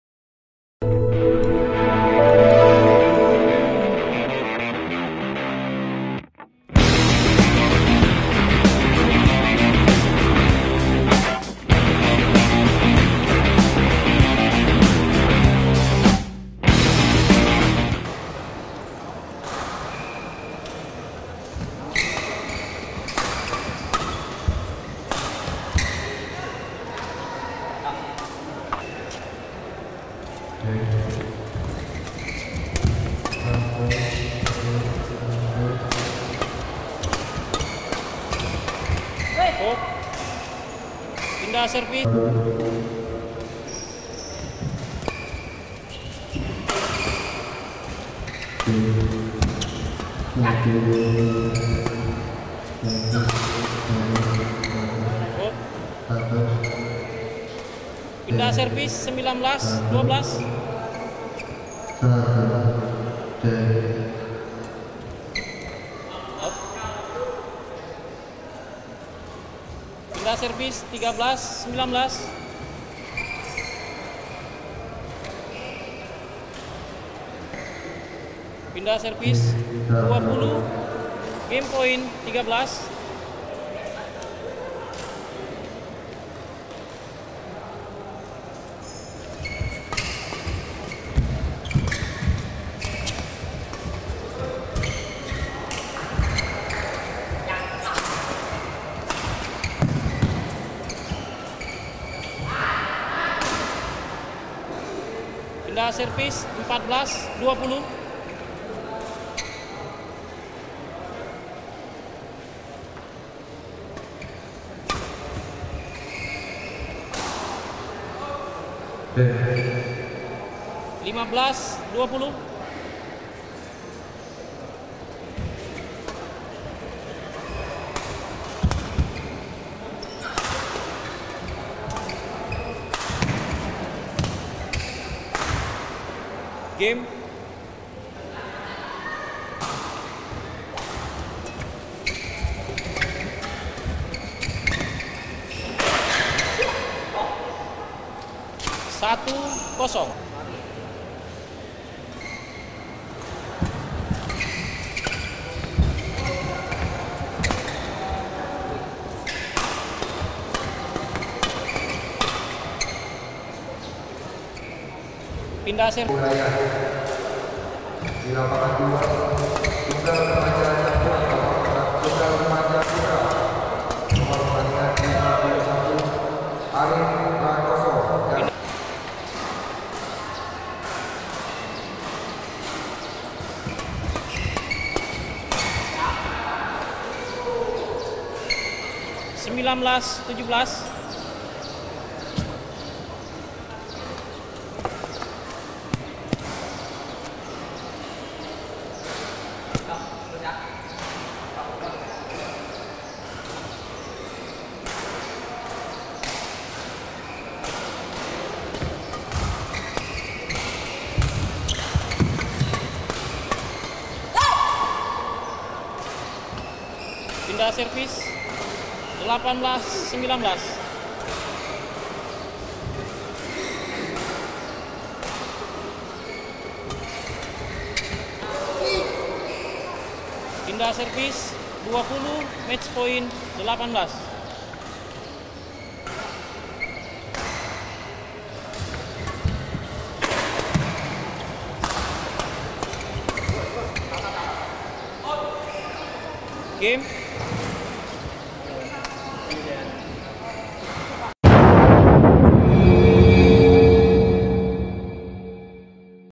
Yohanes Rendy/Dian Fitriani (Pertamina Fastron) VS Fernando K/Marshella Gischa (Djarum Kudus)
Yohanes Rendy/Dian Fitriani (Pertamina Fastron) VS Fernando K/Marshella Gischa (Djarum Kudus) Babak I Ganda Dewasa Campuran Djarum Sirkuit Nasional Kalimantan Barat Open 2015, 28 Sep - 3 Okt 2015, GOR PBSI, Pontianak.